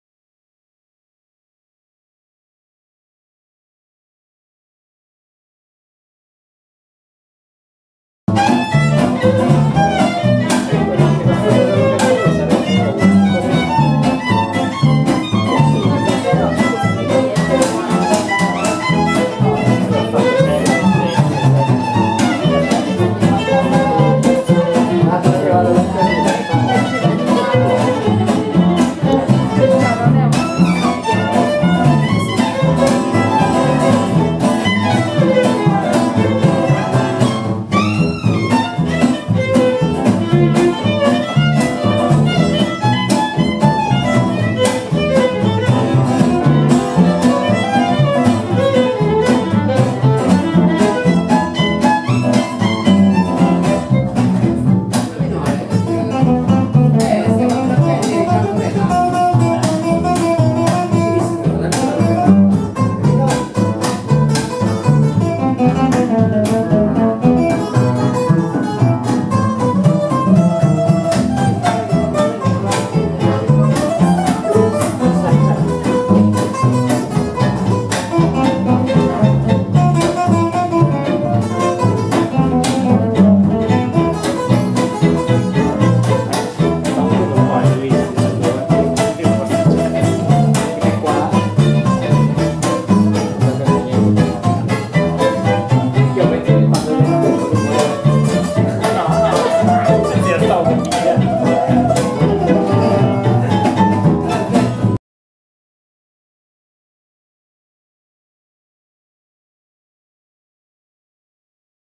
L'officina dell'improvvisazione 1°
prima edizione del Violin Workshop di improvvisazione musicale, presentato da ODOS, a cura di Giovanni Seminerio. Presso il Centro Didattico MondoMusica, direttore artistico Giulia Gangi, per info tel.